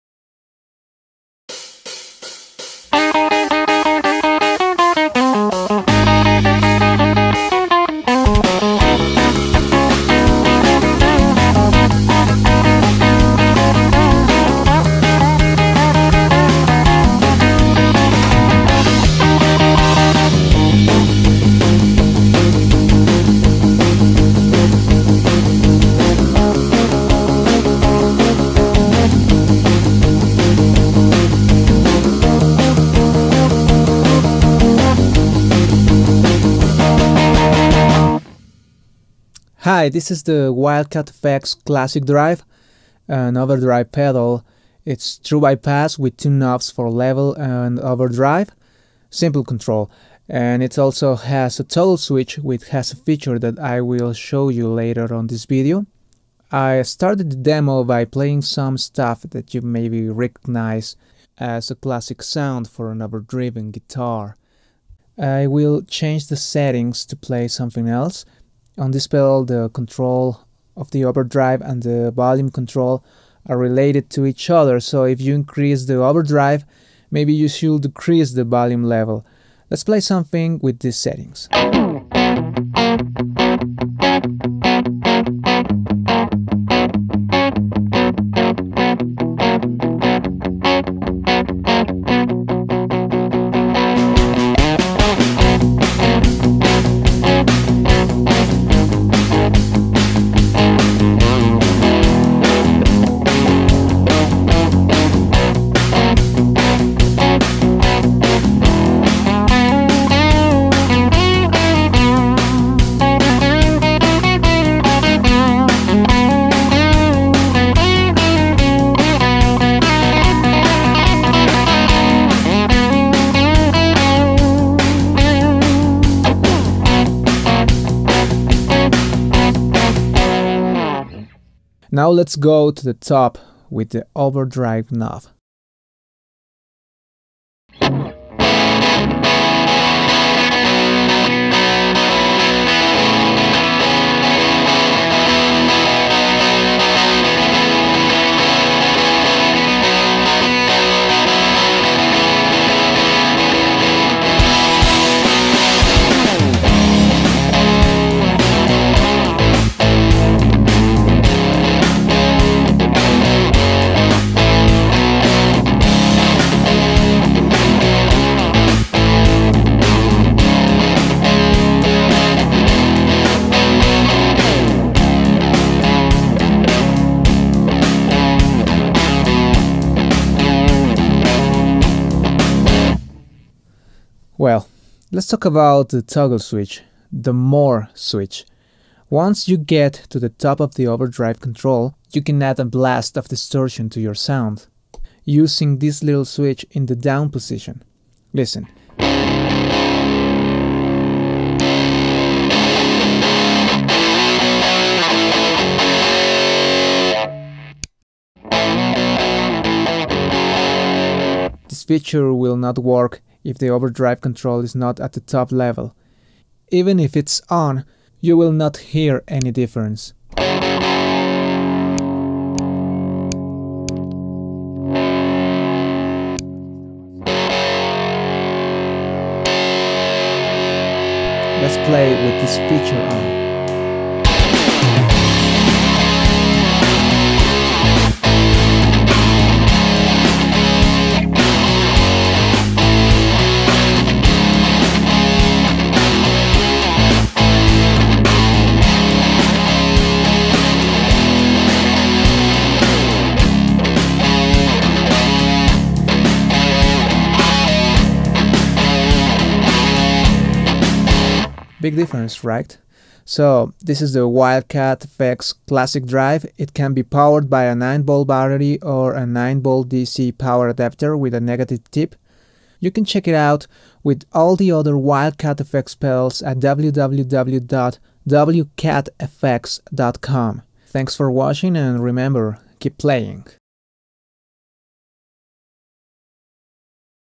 WildCat FX Classic Drive DEMO
Features:

• Two controls; volume and distortion
• Toggle switch boosts distortion
• Heavy duty stomp switch
• Aluminum enclosure
• Red LED
• True bypass
• Uses a 9v battery or 9v DC adaptor (tip negative)